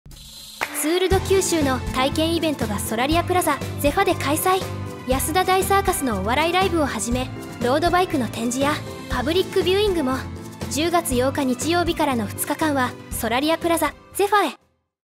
【MP4】ツールド九州イベント告知@ゼファ
以下イベント告知用動画

・催事名：ツールド九州を体験しよう
・日　程：2023年10月8日（日）9日（月祝）
・場　所：ソラリアプラザゼア